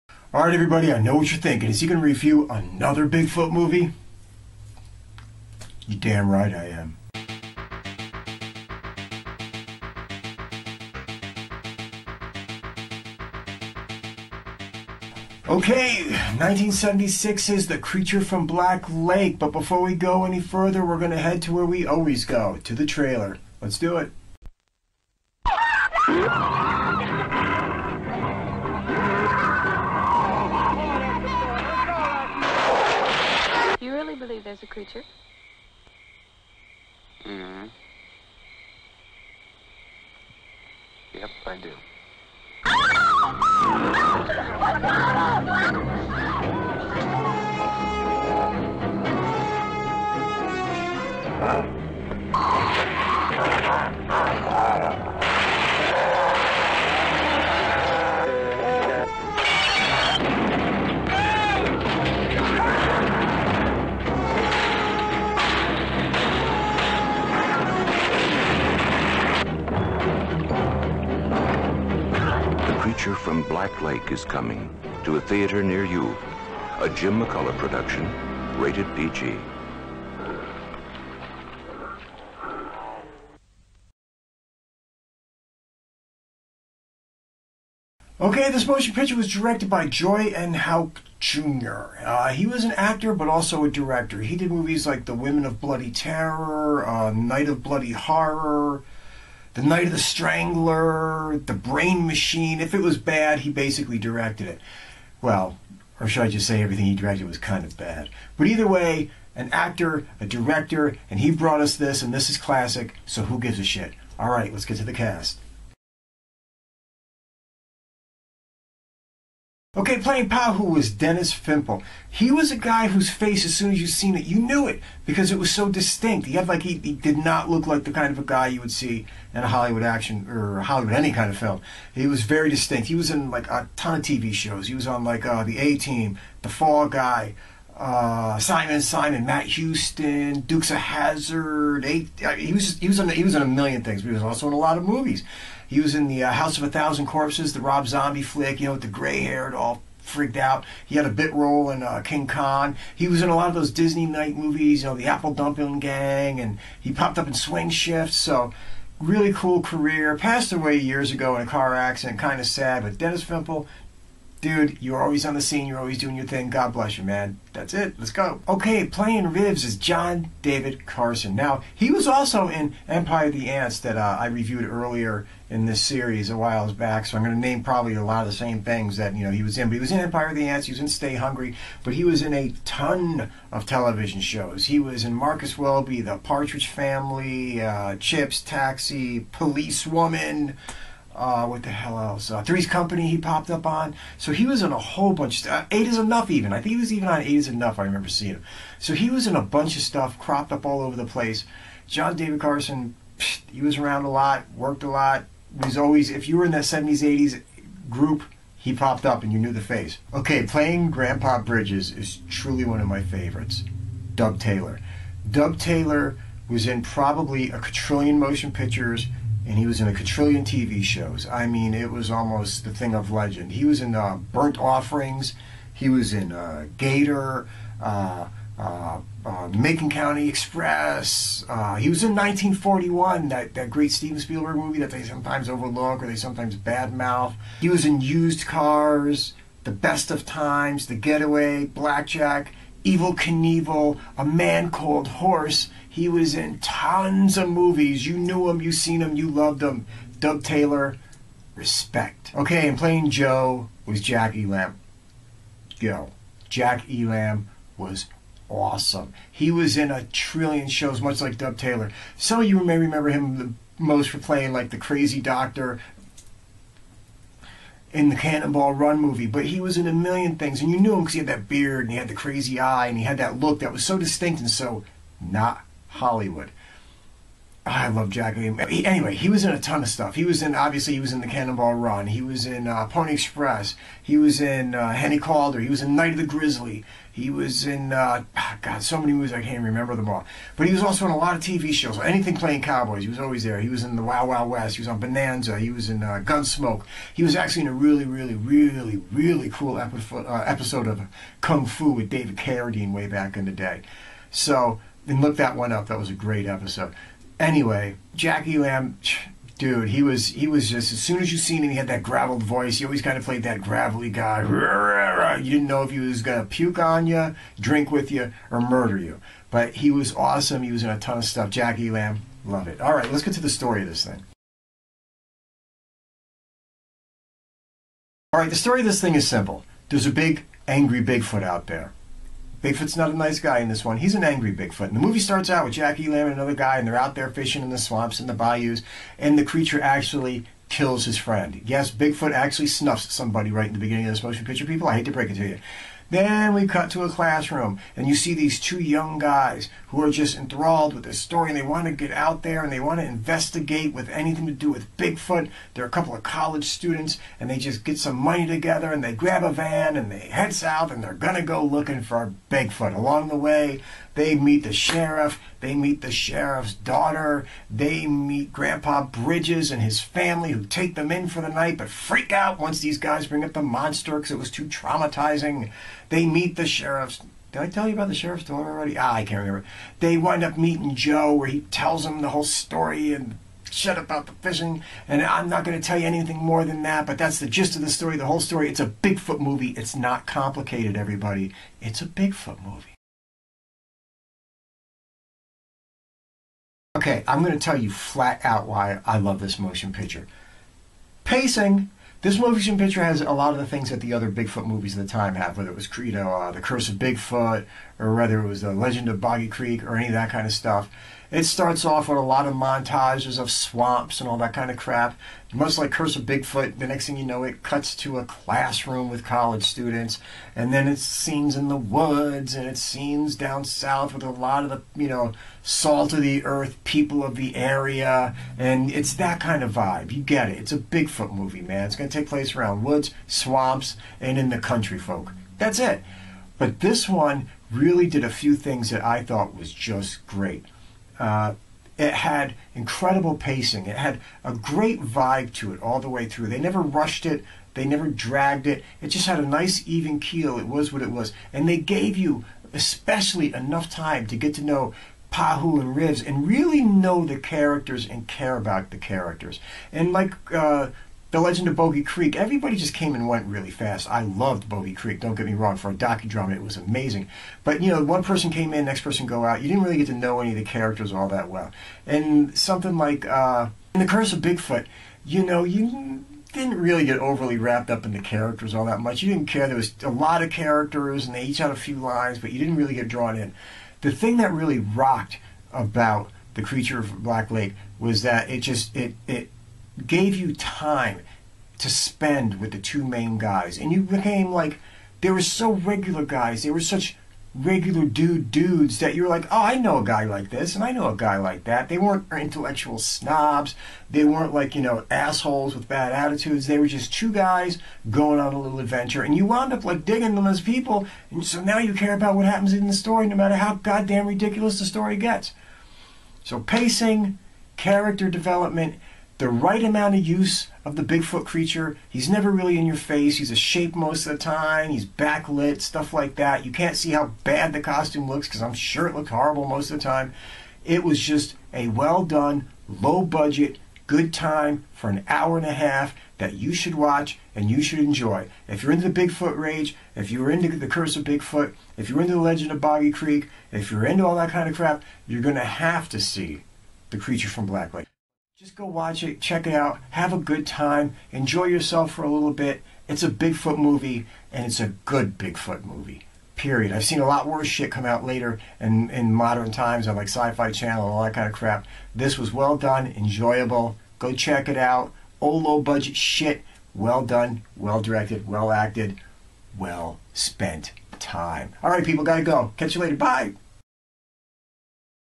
EPISODE 29: "THE CREATURE FROM BLACK LAKE" (1976) REVIEW!!!
Take a look at my review of 1976's The Creature from Black Lake, a true hidden gem.   Enjoy.

"THE CREATURE FROM BLACK LAKE" (1976)

Directed by Joy N. Houck Jr.
Produced by Jim McCullough Jr.   Jim McCullough Sr.
Screenplay by Joy N. Houck Jr.

Jack Elam as Joe Canton
Dub Taylor as Grandpaw Bridges
Dennis Fimple as Pahoo
John David Carson as Rives
Bill Thurman as Sheriff Billy Carter
Roy Tatum as Fred/Creature